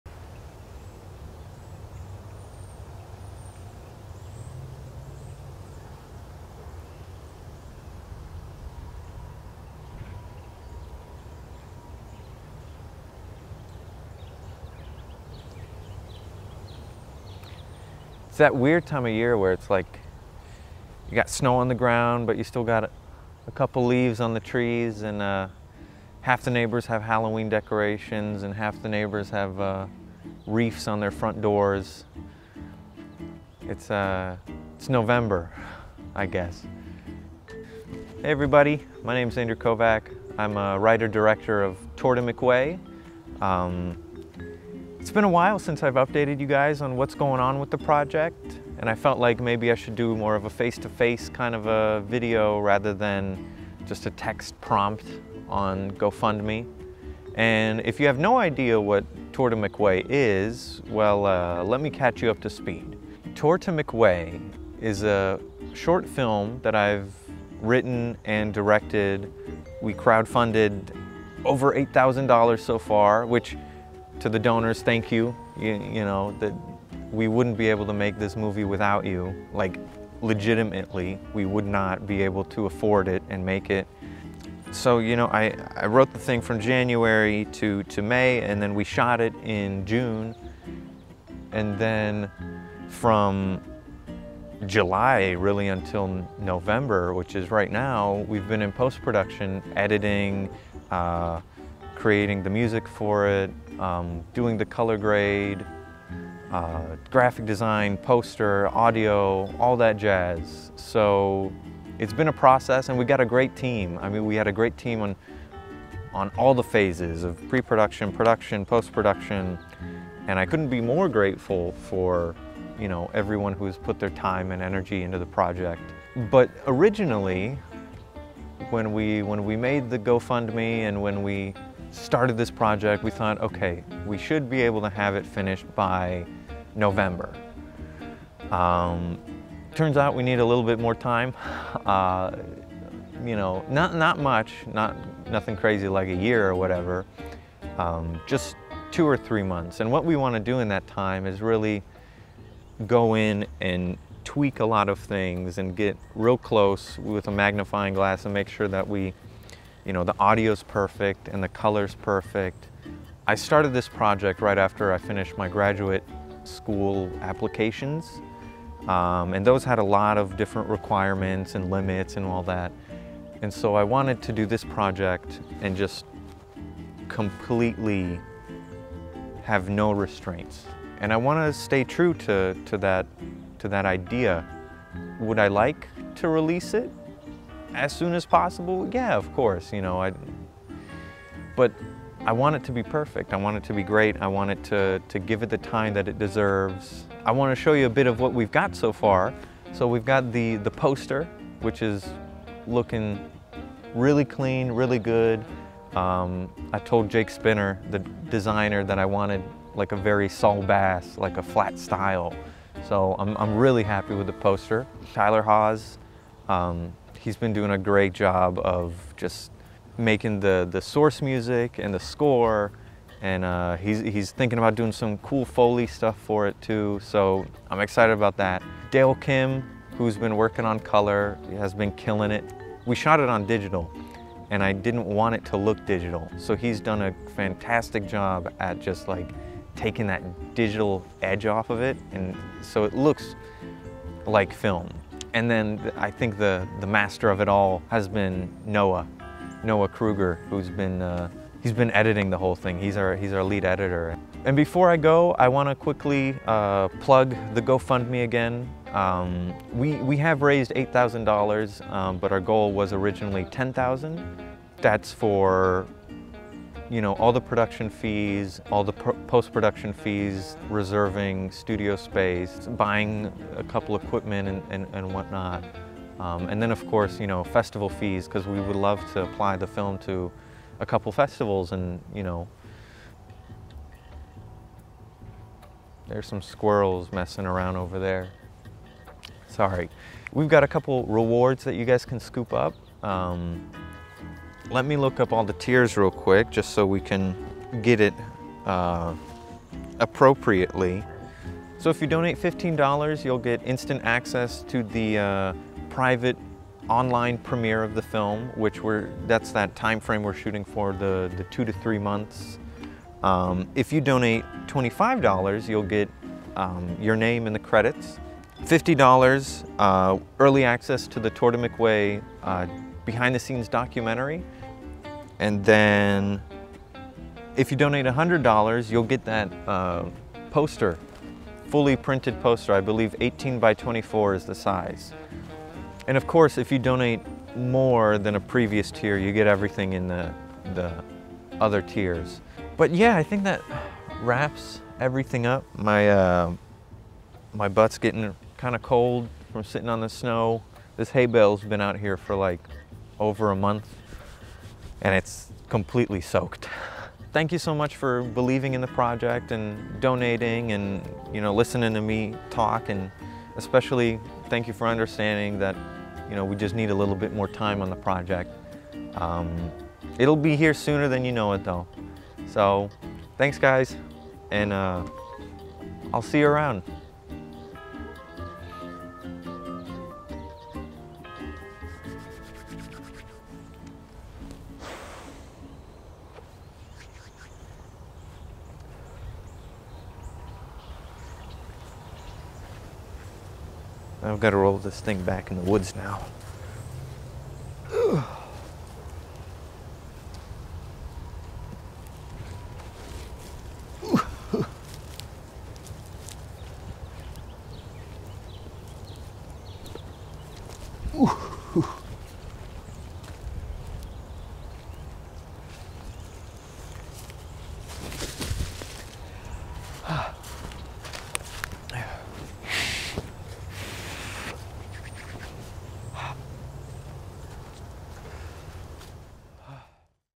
An Update on Tour To McWay
A brief update on the status of my short film, Tour to McWay. Originally we planned for a November release but due to some setbacks, it's going to take longer than expected.

To the donors, thank you for your continued support! Without you, there would be no movie.

If you're interested in becoming a donor or just believe in indie film, you can help us out